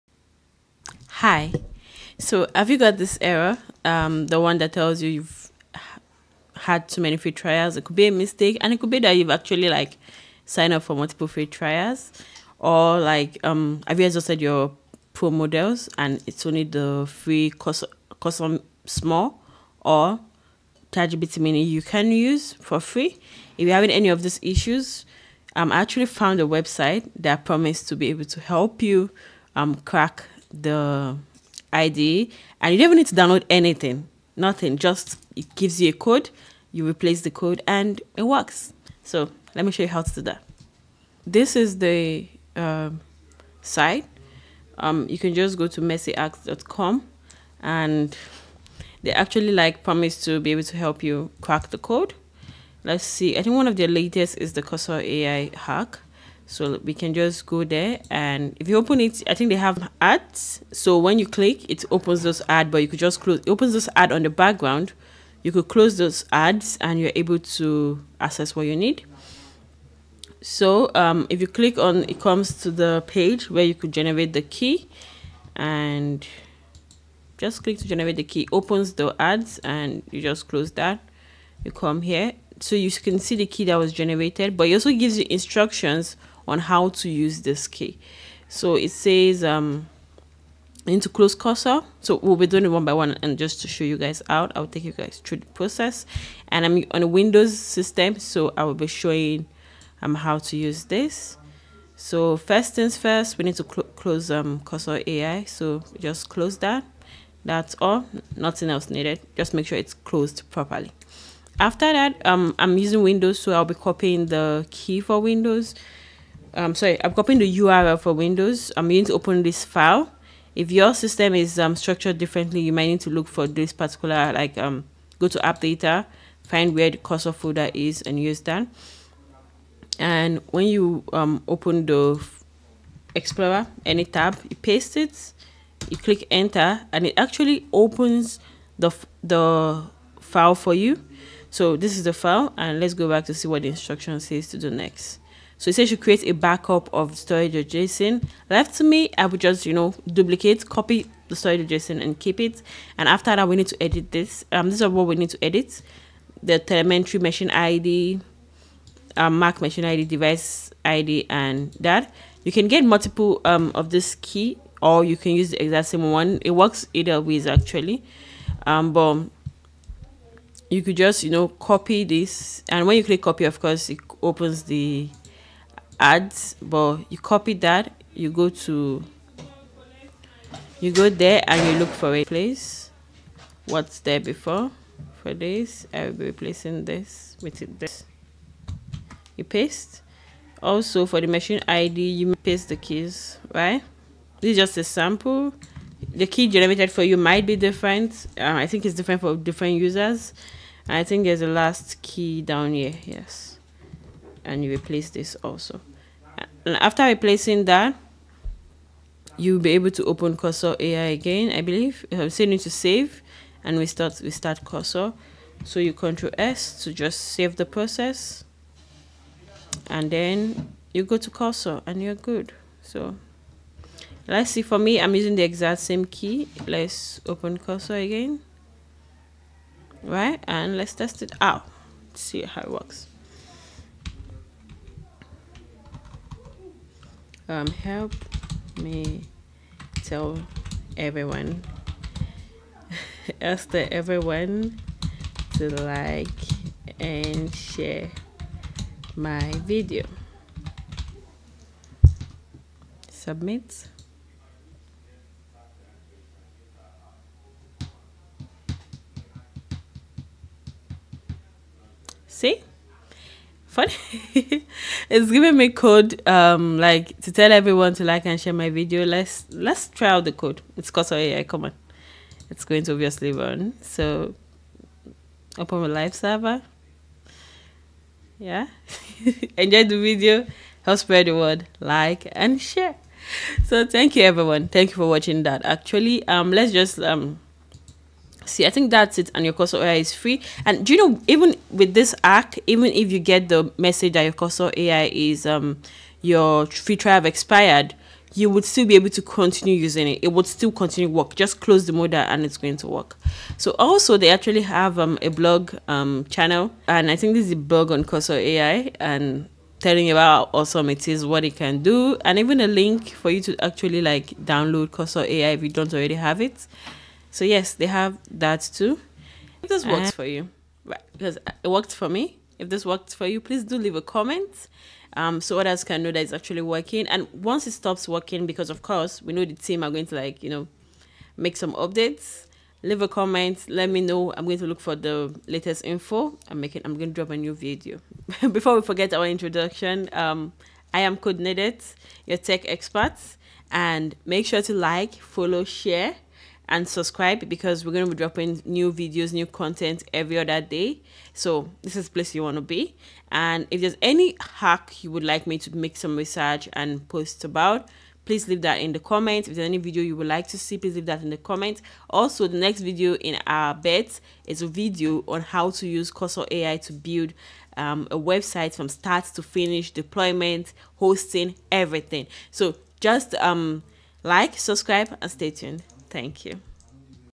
Cursor Ai
In this video we’ll walk you through solving this error above if you paid for cursor but still get the error

TIMESTAMPS
1:37 Generating Cursor AI Free Keys
4:45 Using the keys for unlimited free trial

Our channel is about technology. We cover lots of cool stuff such as AI, Software development and other nerd stuff!